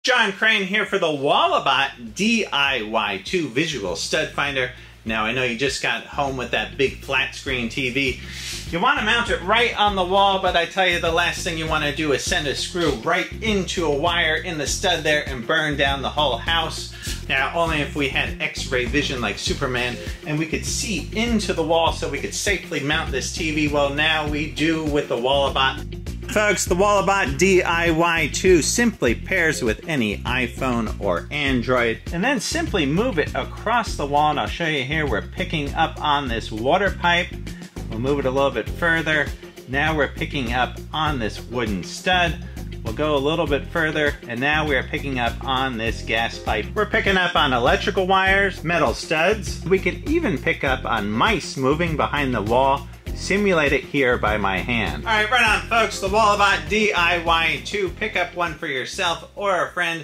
X-RAY Vision - Walabot Stud Finder - See Through Walls🦸‍♂️
Check it out on Amazon

The tool is a cutting-edge device which is paired with your iOS or Android phone to produce a visual image of what is inside drywall up to 4 inches (10 cm) deep.